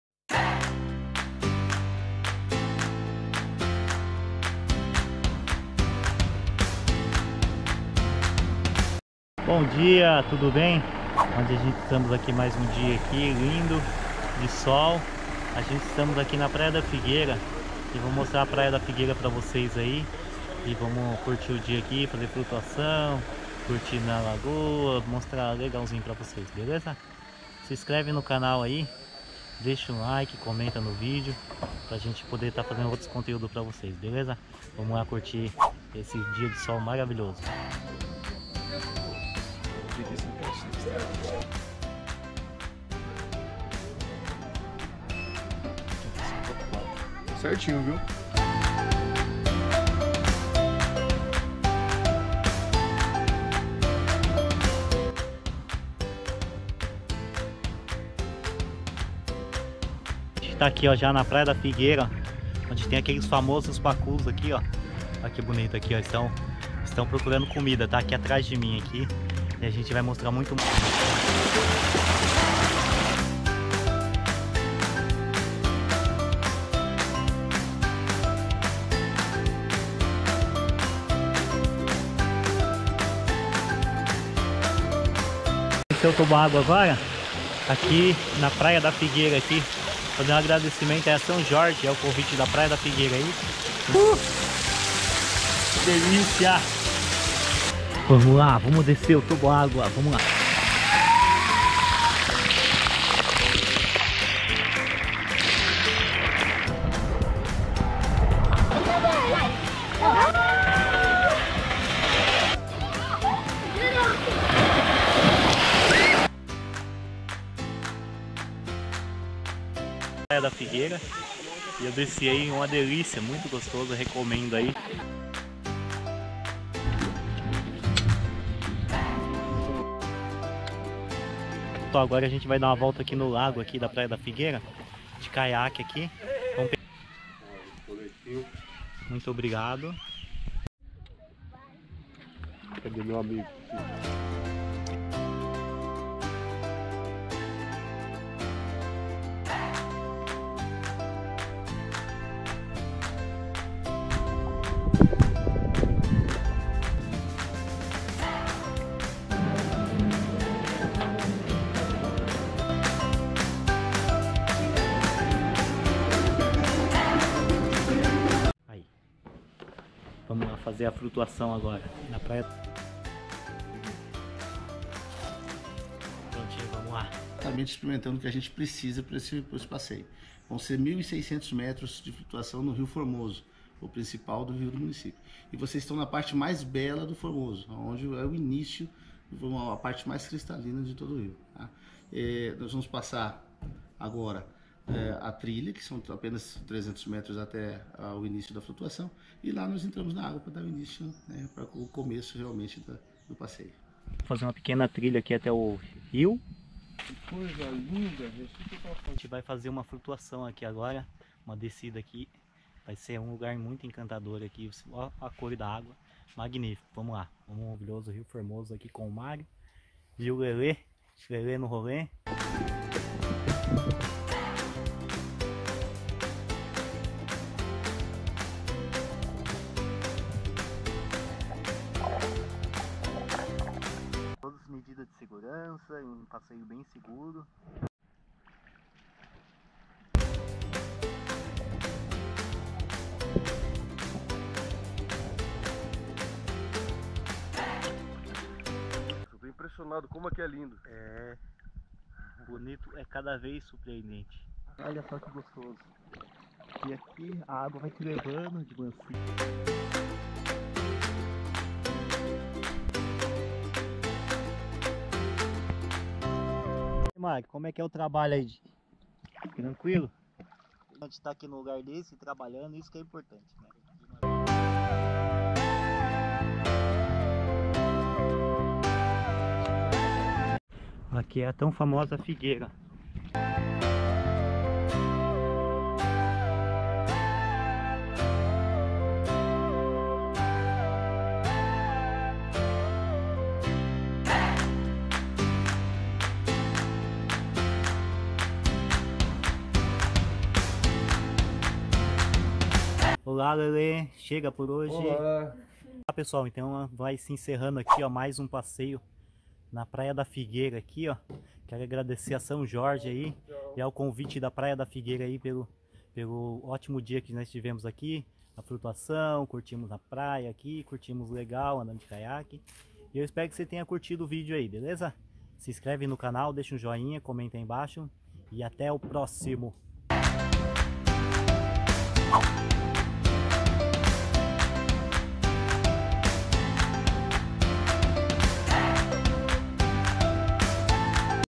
PRAIA DA FIGUEIRA-- BONITO/MS🌳
Balnéario Praia da Figueira Toboágua e Flutuação BONITO MS .
 Lagoa águas calmas, cristalinas e com temperatura agradável o ano inteiro, esse é o cenário que espera por você na flutuação da Praia da Figueira, e agora com o nono Toboágua como atração em Bonito, Mato Grosso do Sul.
Como funciona
A flutuação da Praia da Figueira, ocorre no Rio Formoso, onde os visitantes percorrem 1.000 metros em aproximadamente uma hora de passeio. Não é preciso saber nadar para curtir essa aventura, pois os guias acompanham todo o trajeto, além disso, é disponibilizado um barco de apoio e ocorre uma parada para descanso em um deck no meio da flutuação. Acabando a etapa da flutuação, você pode desfrutar da Praia da Figueira pelo resto do dia
veja também como ir Praia da Figueira em Bonito MS.